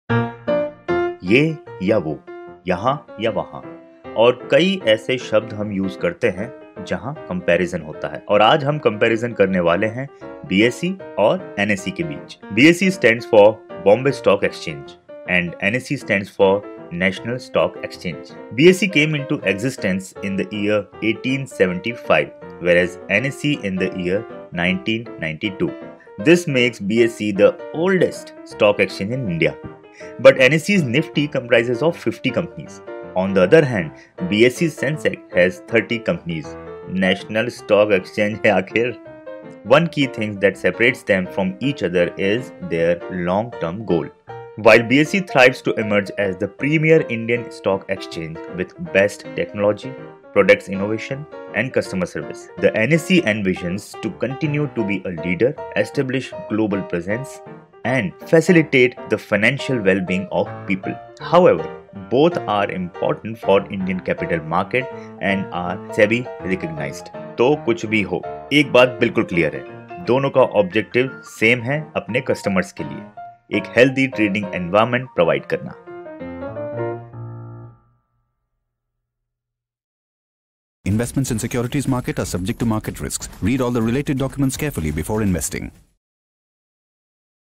What is the Difference Between BSE & NSE? | Angel Broking Knowledge Series
What is the Difference Between BSE & NSE?
BSE stands for the Bombay Stock Exchange whereas NSE stands for National Stock Exchange.

BSE and NSE have organized marketplace where investors and traders transact in shares and securities. We all have come across the word Sensex in the stock market.
BSE came into existence in 1875 & NSE came into existence in 1992

In India, there are two major stock exchanges – NSE or National Stock Exchange & BSE or Bombay Stock Exchange.
BSE is the oldest stock exchange in Asia while NSE is the largest in the country.

Watch this video completely to get a clear understanding of BSE & NSE.